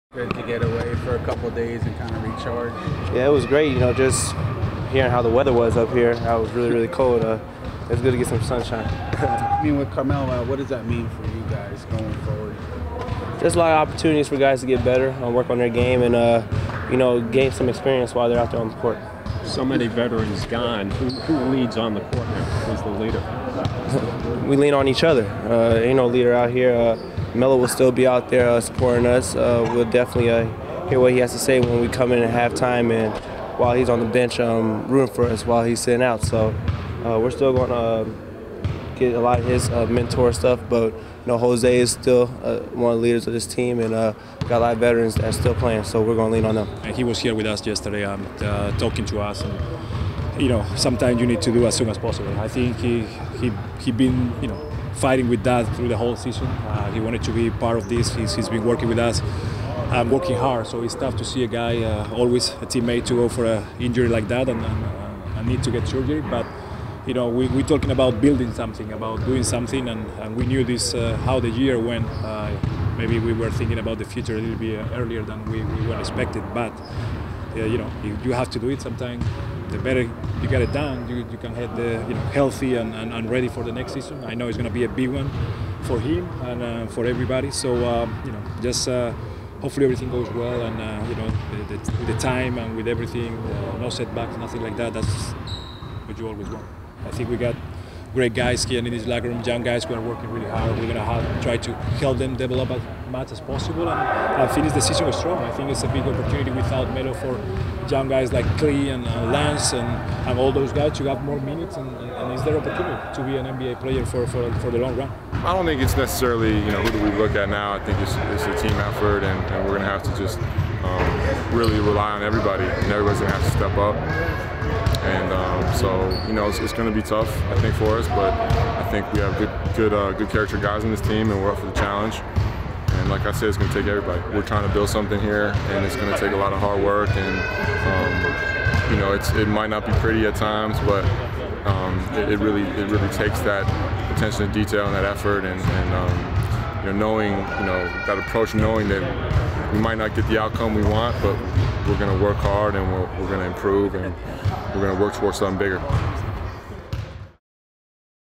THURS Practice: Knicks Prep For Matchup Against Heat
The Knicks prepare for the matchup against the Heat on Friday night.